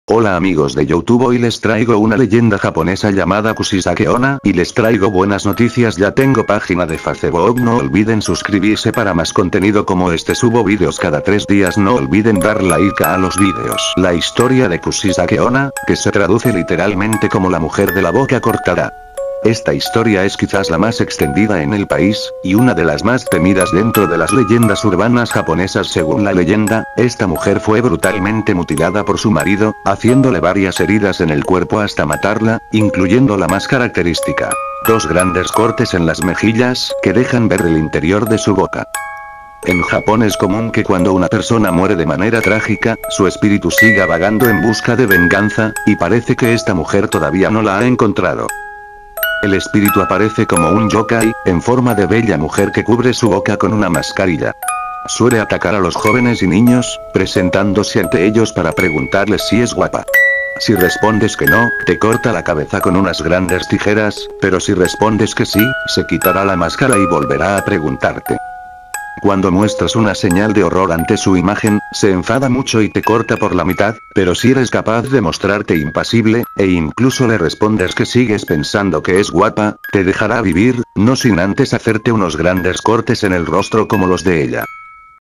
La Leyenda De Kushisake onna
Hola Amigos De YouTube Oy Les Traigo Una Leyenda Japonesa Llamada Kushisake onna